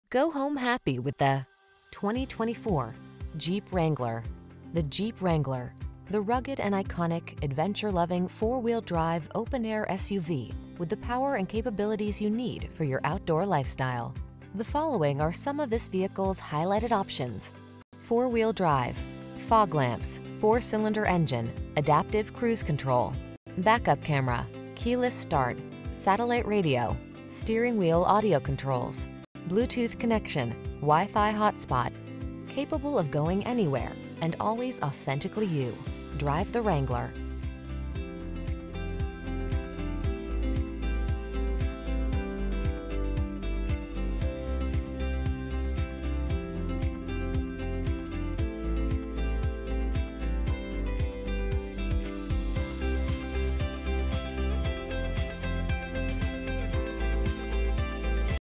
2024 Jeep Wrangler Sport S Atlanta, Marietta, Roswell, Kennesaw, Woodstock GA
New 2024 Jeep Wrangler Sport S available at Ed Voyles CDJR located in 789 Cobb Pkwy SE, Marietta, GA, 30060.
Stock#: 514675 - VIN#: 1C4PJXAN9RW354642

Go home happy with the  2024  Jeep Wrangler. The Jeep Wrangler: The rugged and iconic, adventure-loving, 4-wheel drive, open-air SUV with the power and capabilities you need for your outdoor lifestyle. The following are some of this vehicles highlighted options: Four Wheel Drive, Fog Lamps, 4 Cylinder Engine, Adaptive Cruise Control, Back-Up Camera, Keyless Start, Satellite Radio, Steering Wheel Audio Controls, Bluetooth® Connection, WiFi Hotspot. Capable of going anywhere, and always authentically you. Drive the Wrangler.